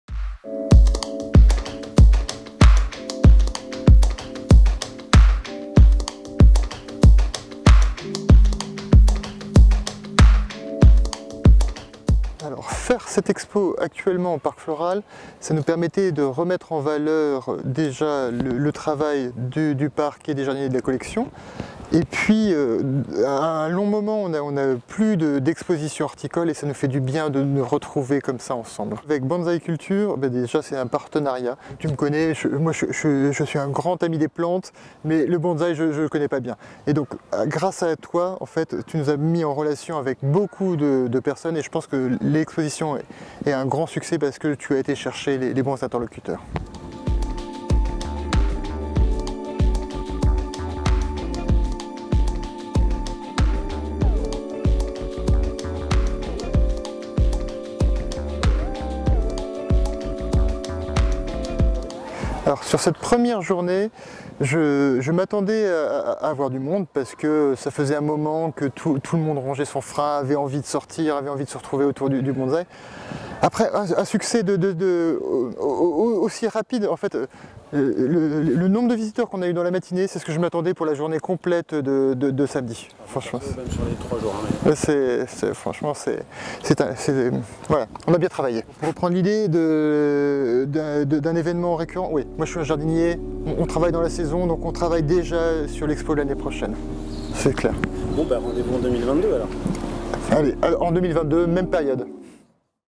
VINCENT LYSIAK BONSAÏ CULTURE AU PARC FLORAL DE PARIS 2021
Vincent Lysiak est responsable du pôle horticole de la division du bois de Vincennes. Il est l'un des architecte de l'événement BONSAÏ CULTURE AU PARC FLORAL DE PARIS 1ére édition. Il nous parle de sa vision de cette événement unique dans la capitale et de son avenir.

BONSAÏ NETWORK